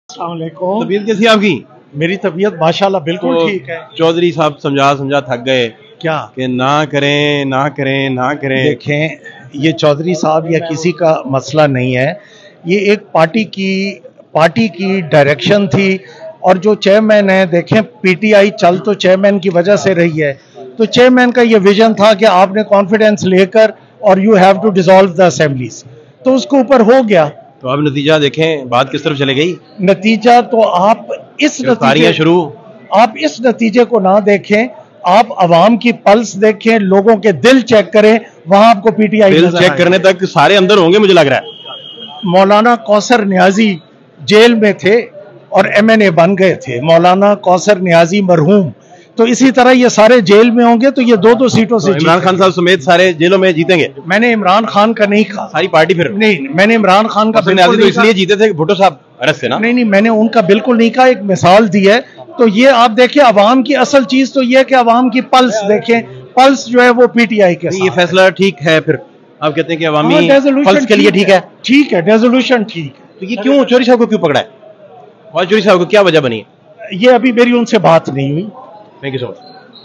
Imran Khan Ko Bhi Jail Mein Dekh Rahe Hain? Anchor Ke Sawal Par Sibtain Khan Ne Kya Kaha
Maulana Kausar Niazi Bhi To Jail Se Jeet Gaye The, Bhutto Sahab To Jail Mein The To Kya Imran Khan Ko Bhi Jail Mein Dekh Rhe Han?

Speaker Punjab Assembly Sibtain Khan Exclusive Talk With Fahad Shahbaz | Dunya News